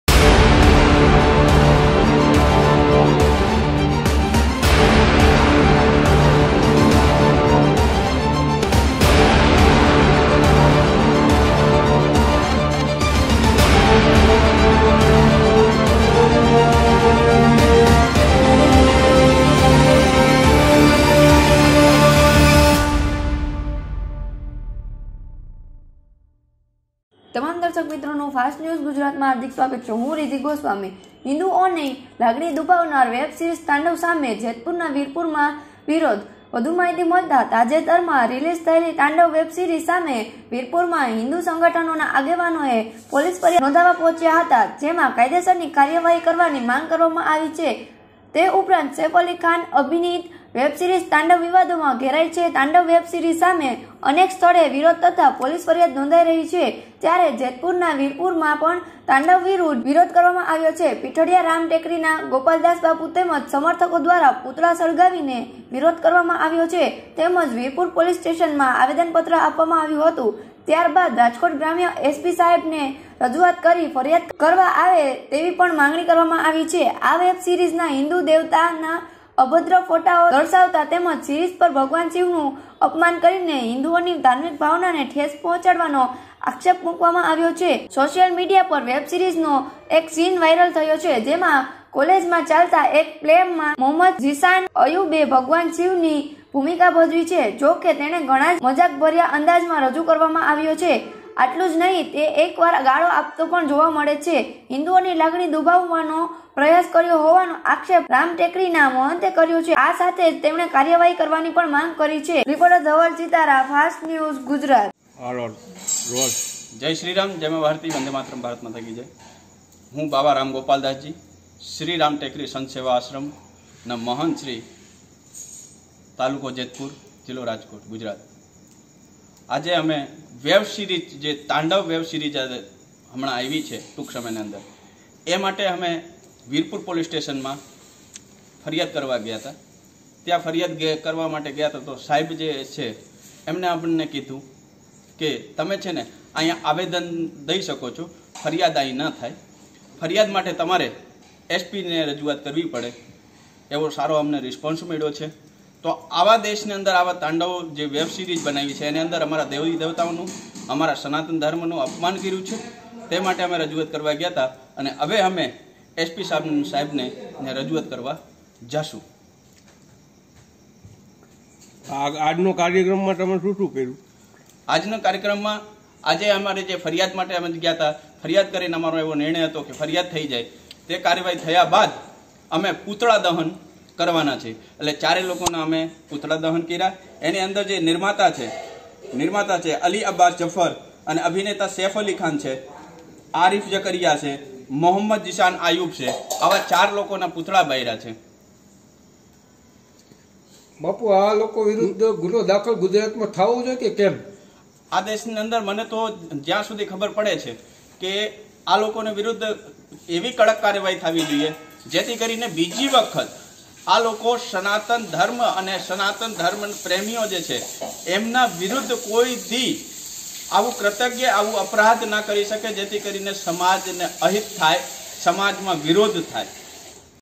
Fast news gujarat
હિન્દુઓ ની લાગણી દુભાવનાર વેબ સિરીઝ ‘તાંડવ’ સામે જેતપુરના વીરપુરમાં વિરોધ

તાજેતરમાં રિલીઝ થયેલી તાંડવ વેબ સિરીઝ સામે વીરપુર માં  હિન્દુ સંગઠનોના આગેવાનોએ પોલીસ ફરિયાદ નોંધવા પહોંચ્યા હતા. જેમાં કાયદેસરની કાર્યવાહી કરવાની માગ કરવામાં આવી છે.

સેફ અલીખાન અભિનીત વેબ સિરિઝ તાંડવ વિવાદોમાં ઘેરાઈ છે. તાંડવ વેબ સિરીઝ સામે અનેક સ્થળે વિરોધ તથા પોલીસ ફરિયાદ નોંધાઈ છે, ત્યારે જેતપુરના વીરપુરમાં પણ તાંડવ વિરુદ્ધ વિરોધ કરવામાં આવ્યો છે. પીઠડીયા રામ ટેકરી ના ગોપાલદાસ બાપુ તેમજ સમર્થકો દ્રારા પૂતળા સળગાવી ને વિરોધ કરવામાં આવ્યો તેમજ વીરપુર પોલીસ સ્ટેશનમાં આવેદનપત્ર આપવામાં આવ્યું હતું ત્યાર બાદ રાજકોટ ગ્રામ્ય એસપી સાહેબ ને રજુઆત કરી ફરિયાદ કરવા આવે તેવી પણ માંગણી કરવામાં આવી છે.
આ વેબ સિરીઝમાં હિન્દુ દેવતાના અભદ્ર ફોટાઓ દર્શાવતાં તેમજ  સિરીઝ પર ભગવાન શિવનું અપમાન કરીને હિંદુઓની ધાર્મિક ભાવનાને ઠેસ પહોંચાડવાનો આક્ષેપ મૂકવામાં આવ્યો
સોશિયલ મીડિયા પર વેબ સિરીઝનો એક સીન વાઈરલ થયો છે, જેમાં કોલેજમાં ચાલતા એક પ્લેમાં મોહમ્મદ ઝીશાન અયુબે ભગવાન શિવની ભૂમિકા ભજવી છે. જોકે, તેને ઘણાં જ મજાકભર્યા અંદાજમાં રજૂ કરવામાં આવ્યો છે. આટલું જ નહીં તે એકવાર ગાળો આપતો પણ જોવા મળે છે. હિન્દુઓની લાગણી દુભાવવાનો પ્રયાસ કર્યો હોવાનો આક્ષેપ રામ ટેકરીના મહંતે કર્યો છે. આ સાથે જ તેમણે કાર્યવાહી કરવાની પણ માગ કરી છે..
ધવલચિતારા